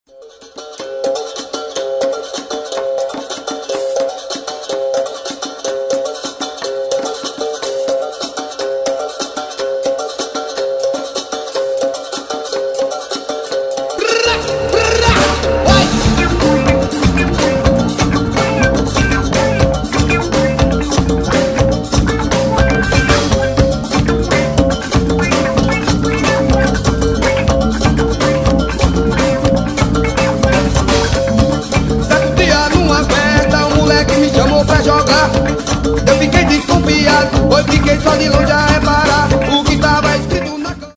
old Capoeira Carioca: Rubberlegs Rasteira
Even standing Carioca Rasteira(see book one) had a more powerful (RubberLegs) version.

Take care when practicing, as this Rasteira can kill.